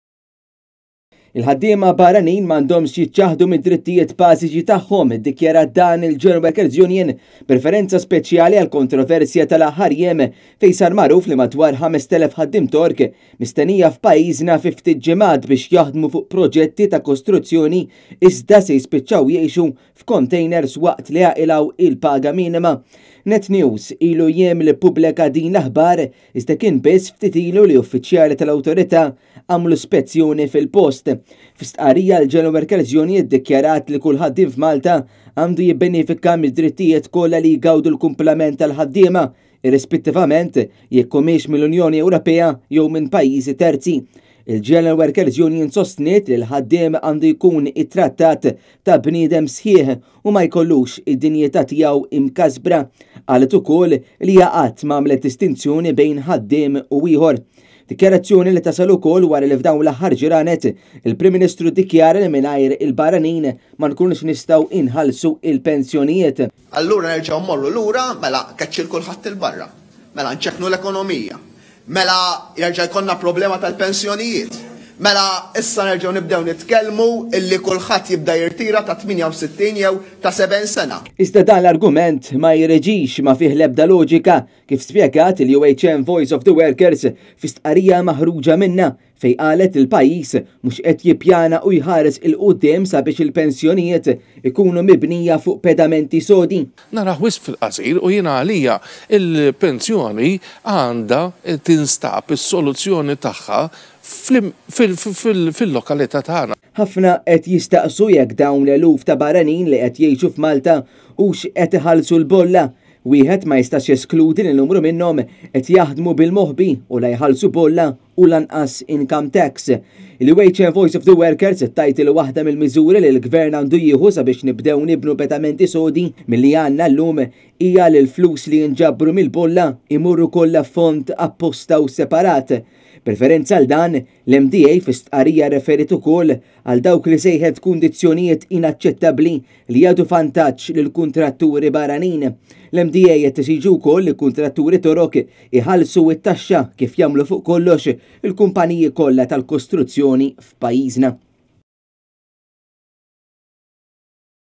Ħtieġa ta’ kundizzjonijiet xierqa għall-ħaddiema Torok – GWU GWU Kostruzzjoni sat 23rd Feb
Iddikajrat dan il-General Workers’ Union b’riferenza speċjali għall-kontroversja tal-aħħar jiem fejn sar magħruf li madwar 5,000 ħaddiem Tork mistennija f’pajjiżna fi ftit ġimgħat biex jaħdmu fuq proġetti ta’ kostruzzjoni iżda se jispiċċaw jgħixu f’kontejners waqt li jaqilgħu l-paga minima.